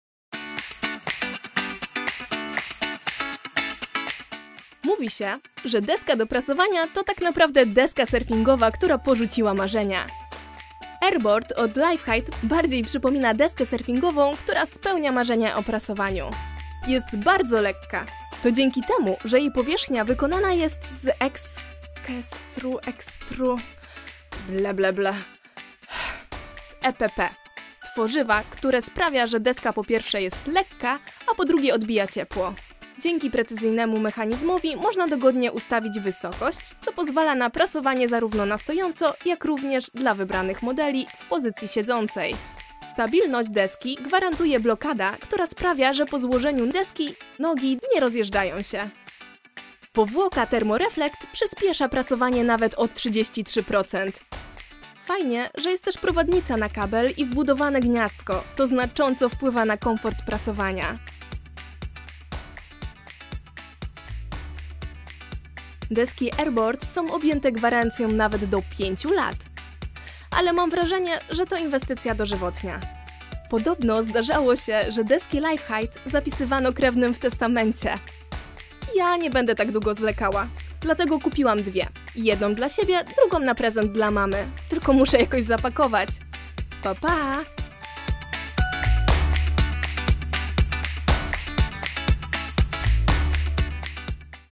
AirBoard - Leifheit - Ultralekka deska do prasowania
Miłośnikom komfortu i oszczędności czasu z pewnością przypadnie do gustu linia AirBoard. Ultra lekka (waga desek
w zależności od modelu zaczyna się od 3 kg), a jednak bardzo stabilna deska spowoduje rewolucję w Twoim prasowaniu.
Zwykłe deski są metalowe, a więc ciężkie. Pokryte gąbką i pokrowcem, wchłaniającymi parę. W deskach AirBoard powierzchnia jest wykonana z ekstrudowanego polipropylenu (EPP) – który tę parę odbija, co w połączeniu z unikalnym pokrowcem deski z powłoką Thermo Reflect, daje prasowanie szybsze nawet o 33%.
Deski AirBoard firmy LEIFHEIT to jedyne deski z powierzchnią do prasowania z tworzywa sztucznego dostępne na polskim rynku. Dzięki precyzyjnemu mechanizmowi można dogodnie ustawić wysokość, co pozwala na prasowanie zarówno na stojąco, jak również w pozycji siedzącej (dla wybranych modeli). Stabilność deski gwarantuje blokada, która sprawia, że po złożeniu nogi deski nie „rozjeżdżają się”. Perfekcyjne wyprasowanie ramion w koszulach, czy T-shirtach zapewnia specjalne profilowanie. Wybrane modele z linii są wyposażone w gniazdka oraz prowadnice do kabla od żelazka bądź stacji parowej. Deski AirBoard są objęte gwarancją nawet do 5 lat.